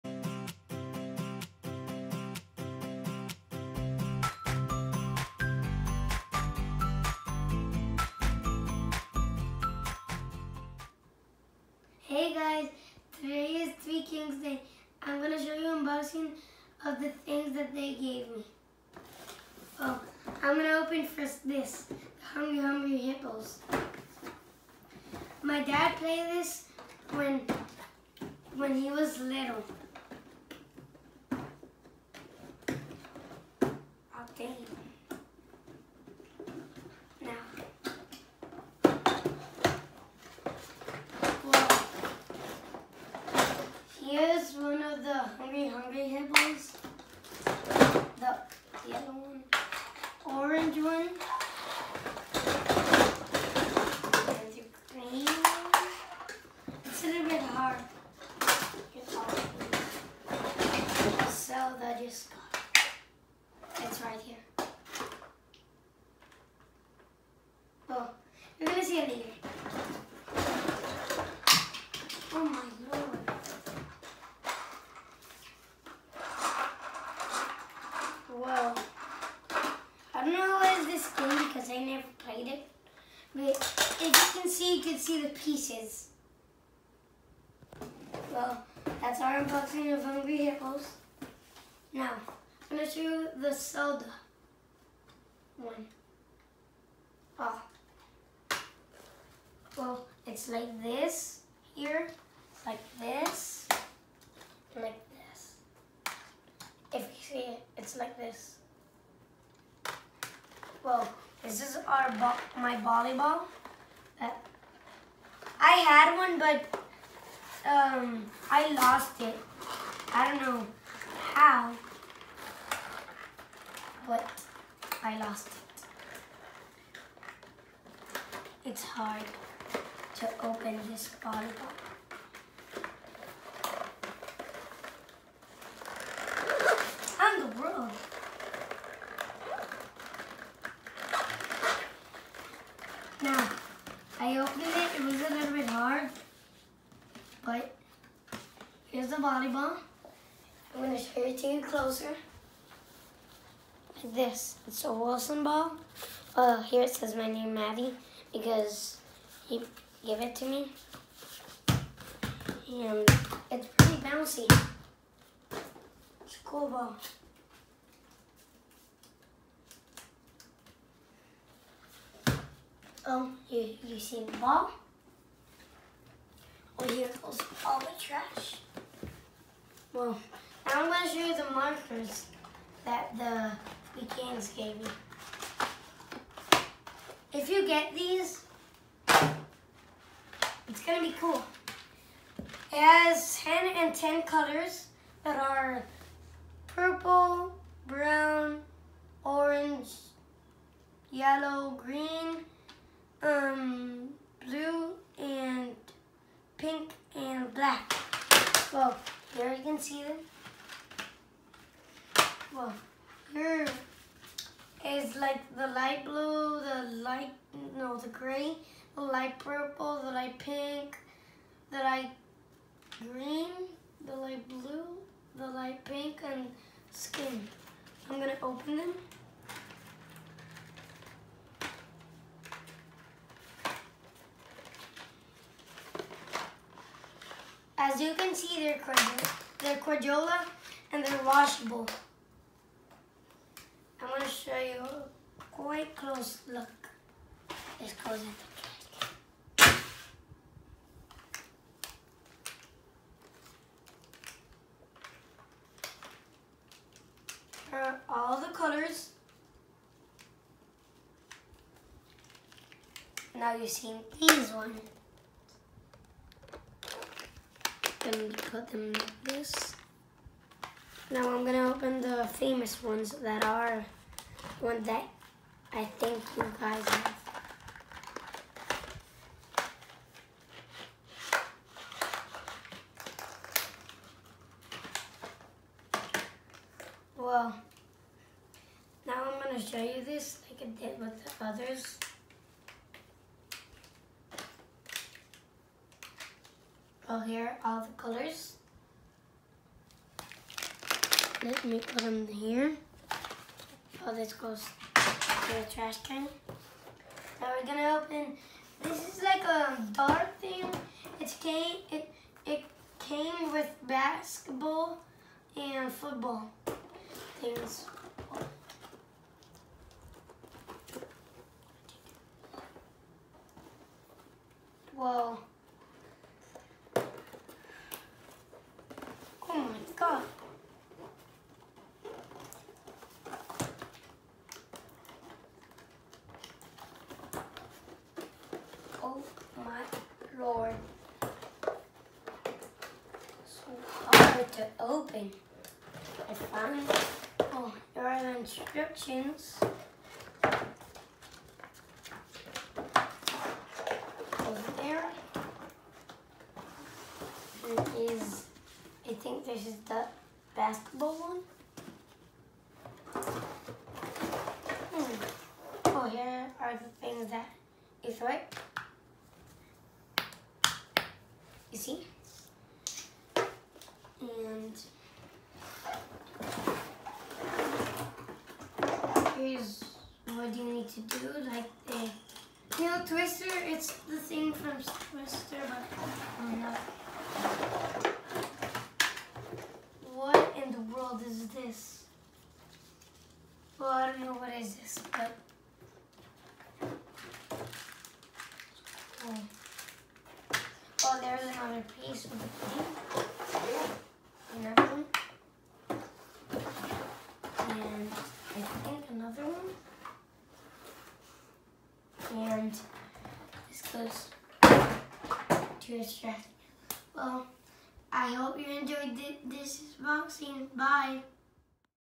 Zelda, Volleyball Ball, Darts Game, Washable Markers and Hungry Hungry Hippos Unboxing
My Three Kings' Day Gifts (Mis regalos de Los Tres Reyes Magos):

The Legend of Zelda “Breath of the Wild” Game for Nintendo Switch

Wilson SoftPlay Volleyball Ball

Hungry Hungry Hippos

Majik Oversized 2-in-1 Football & Basketball Darts

Crayola Ultra-Clean Washable Markers